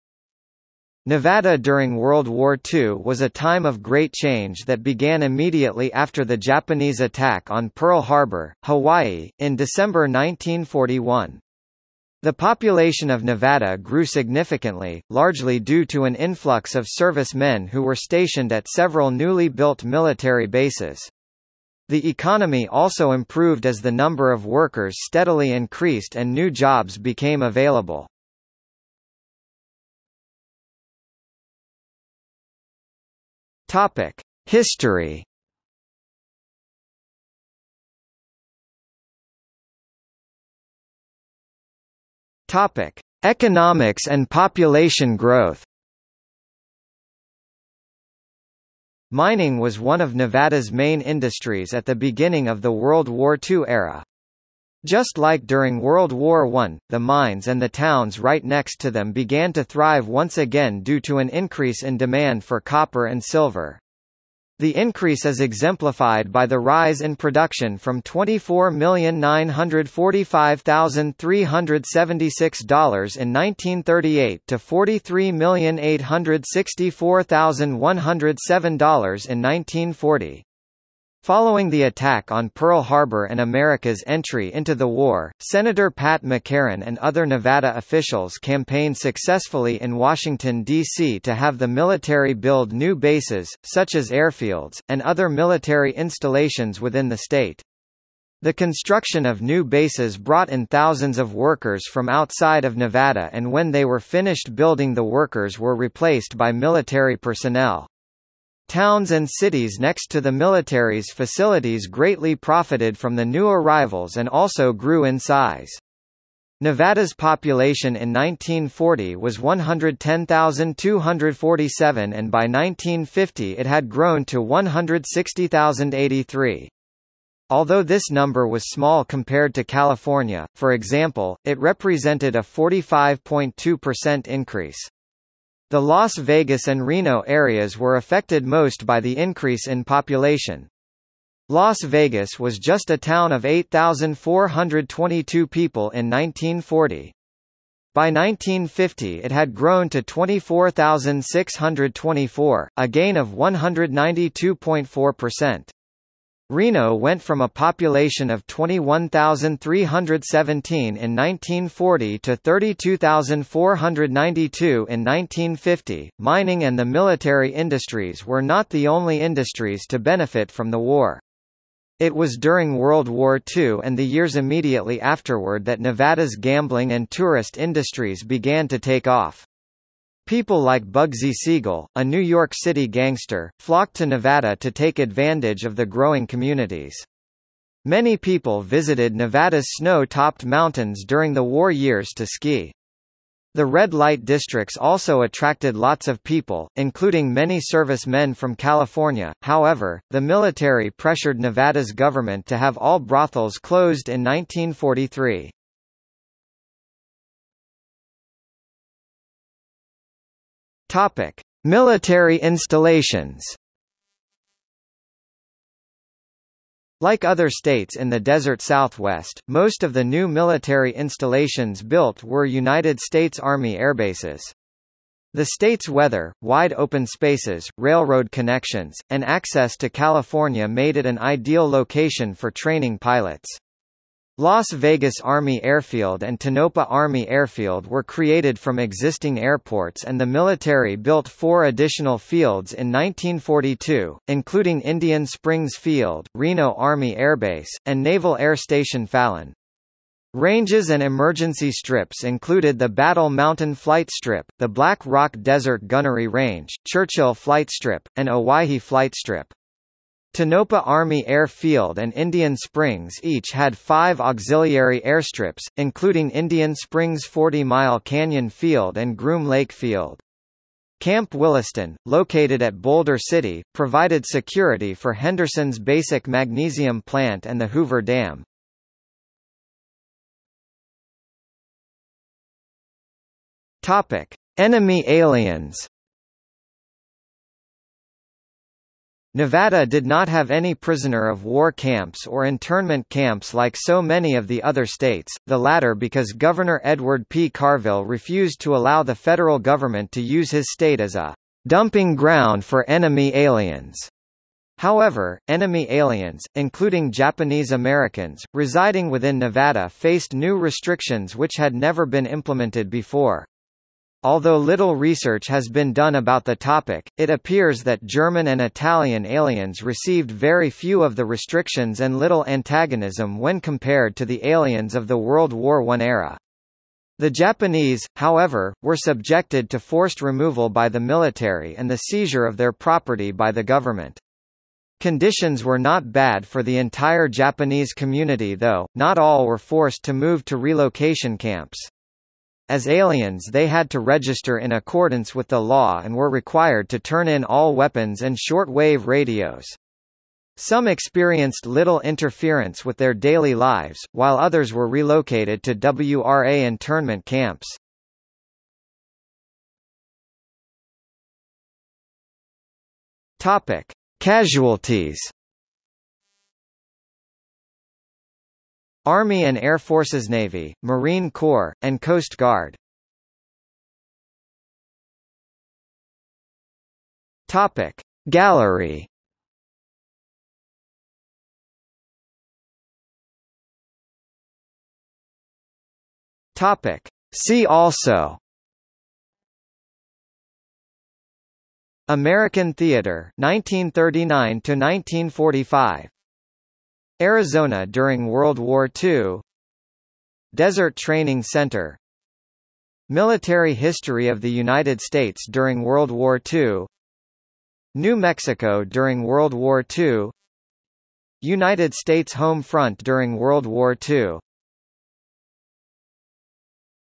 Nevada during World War II | Wikipedia audio article
00:00:30 1 History
00:00:39 1.1 Economics and population growth
00:03:27 1.2 Military installations
00:04:40 1.3 Enemy aliens
00:05:59 2 Casualties
00:06:13 3 Gallery
00:06:22 4 See also

- Socrates

SUMMARY
Nevada during World War II was a time of great change that began immediately after the Japanese attack on Pearl Harbor, Hawaii, in December 1941. The population of Nevada grew significantly, largely due to an influx of service men who were stationed at several newly built military bases. The economy also improved as the number of workers steadily increased and new jobs became available.